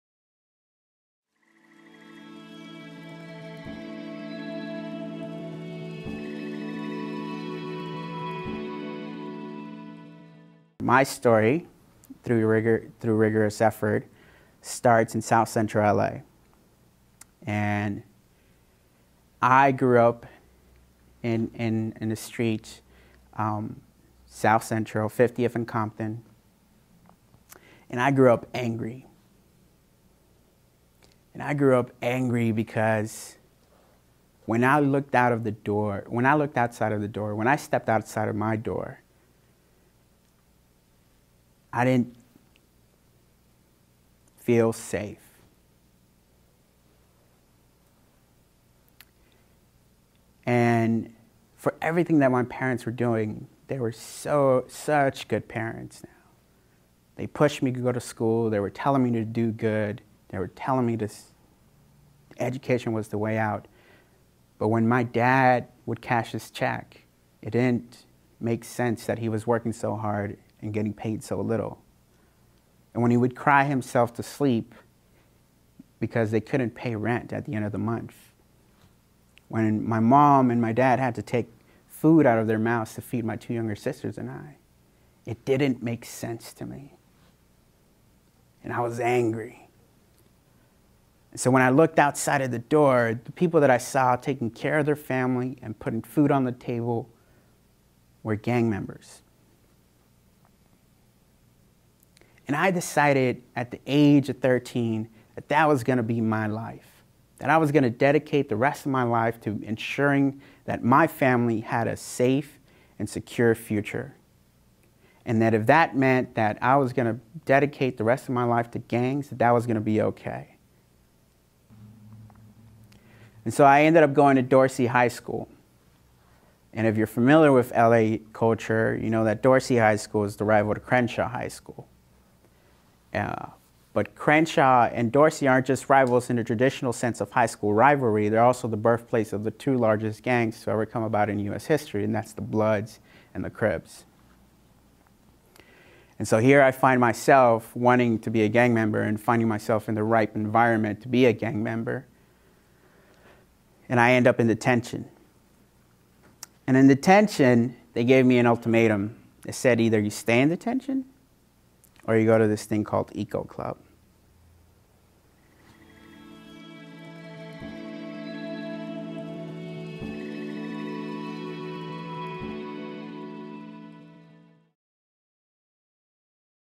Juan Martinez Video: My Story
Manzanita Speaker Series presents Juan Martinez, "Exploring the Urban Wild". National Geographic Explorer, TED Speaker, and a proud product of south central Los Angeles, Juan D. Martinez leads us in a discussion about urban communities, technology, and nature.